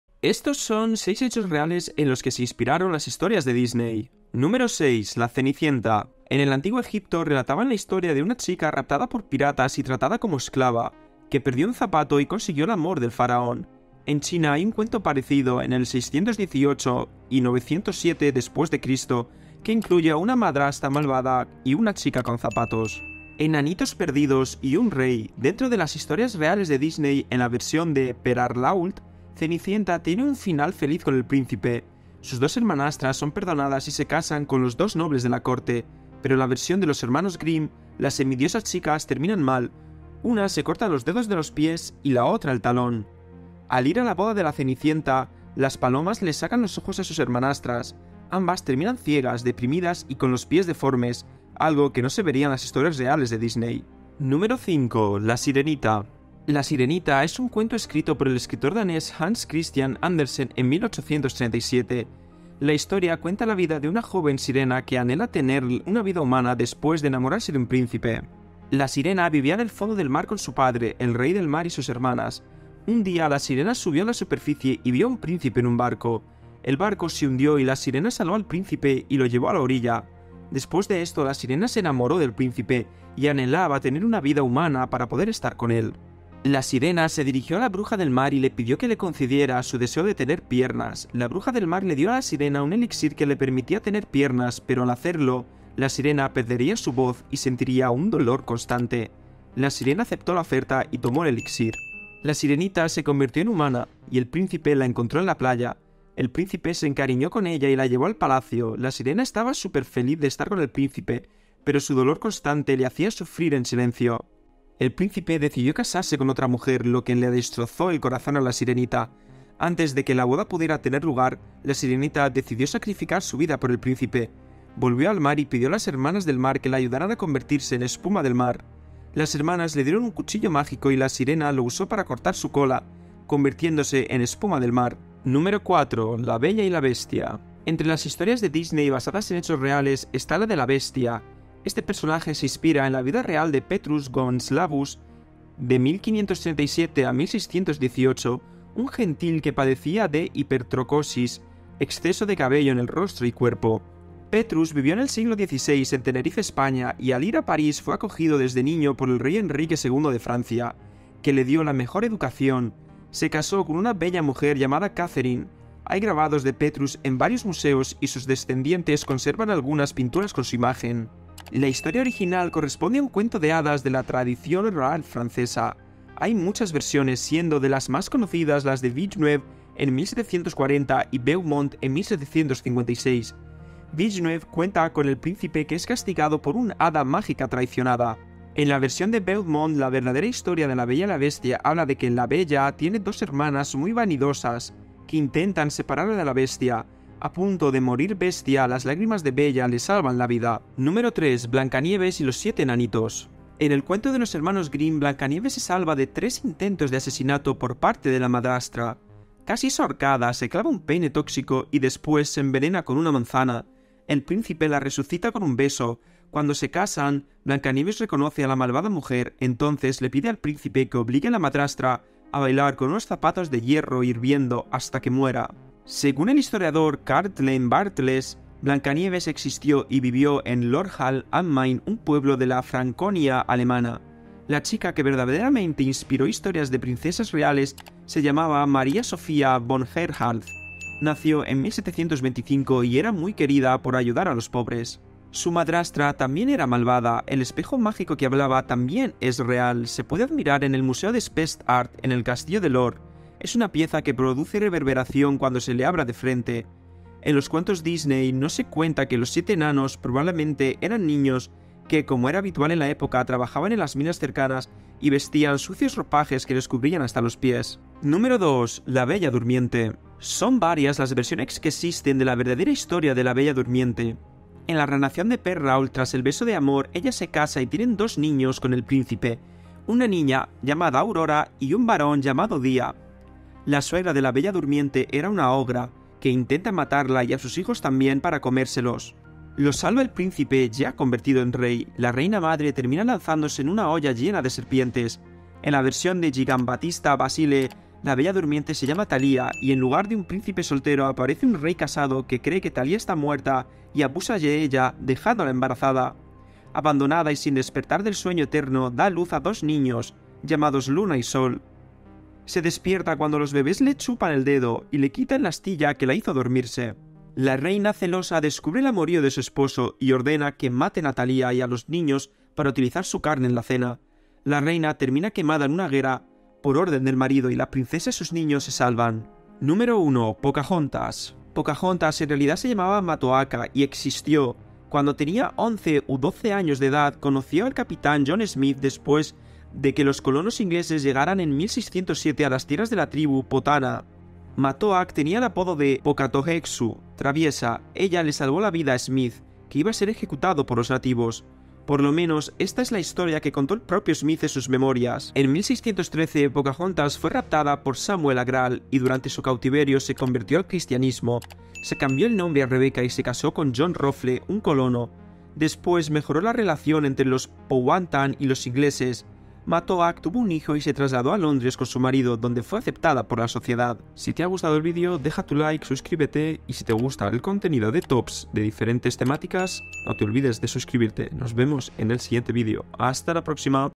HISTORIAS REALES DE DISNEY QUE ARRUINARAN TU INFANCIA
Lastimosamente espero no destruir tu infancia en este video, contándote que en realidad las historias originales de las películas de Disney, eran muy distintas a como las adaptaron en la pantalla grande… Exactamente eso veremos el día de hoy, las historias oscuras y turbias que luego fueron retocadas para los más pequeños